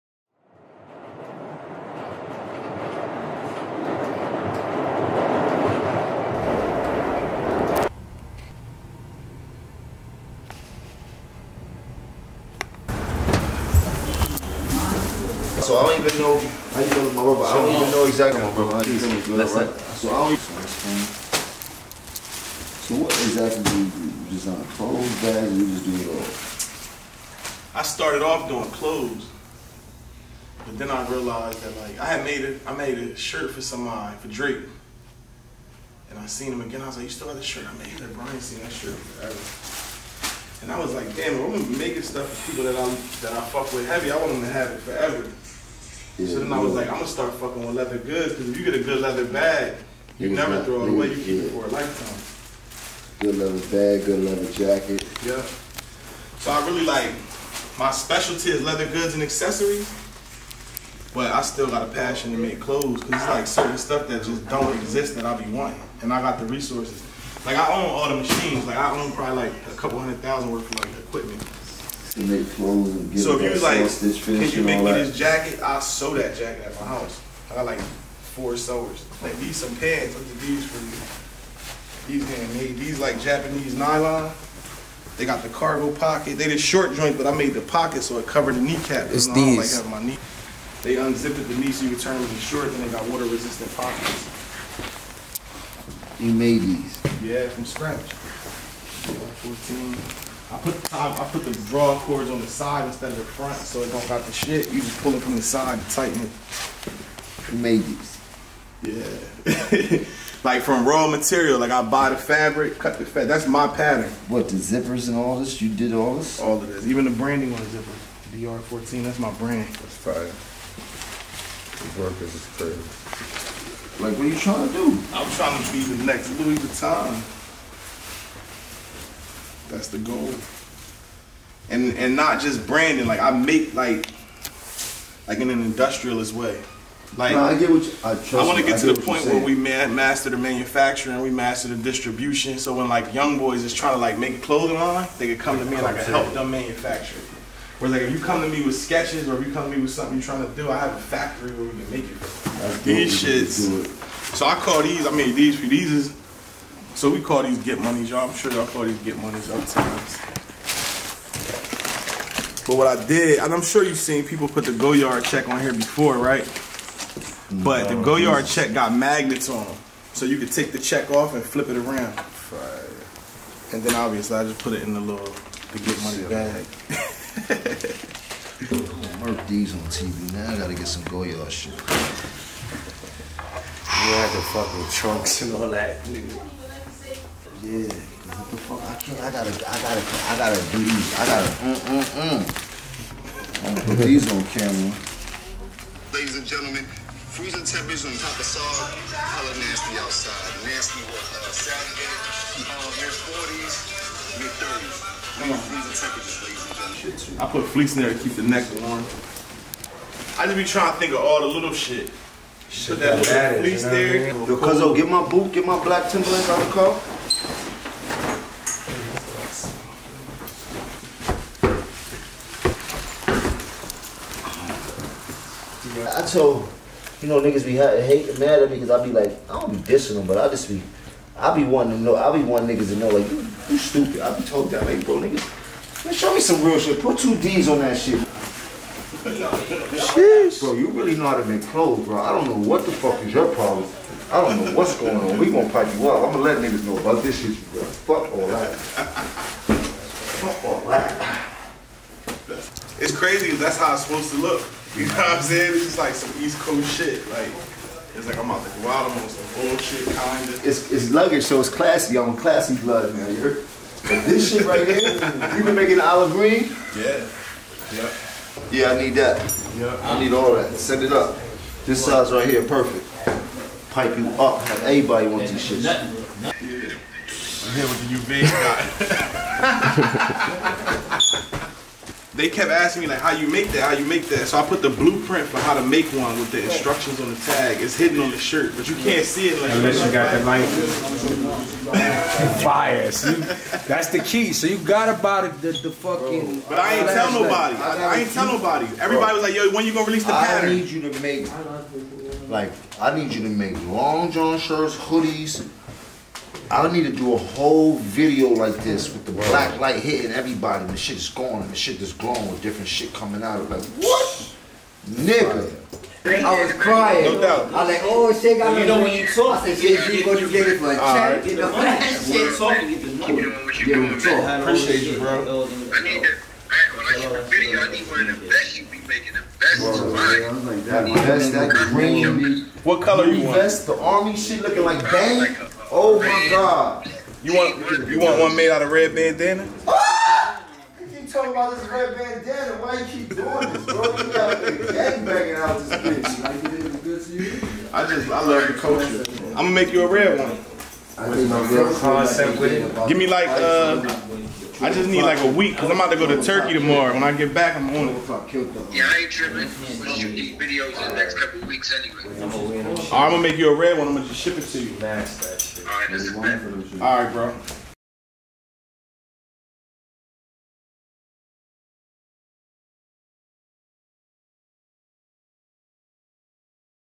Sheron Barber Makes Custom Goyard Air Force 1 For Jim Jones & Juelz Santana (Official Video)
Los Angeles based fashion Designer Sheron Barber delivers custom Goyard Air Force 1 to Jim Jones & Juelz Santana in the heart Of Harlem New York.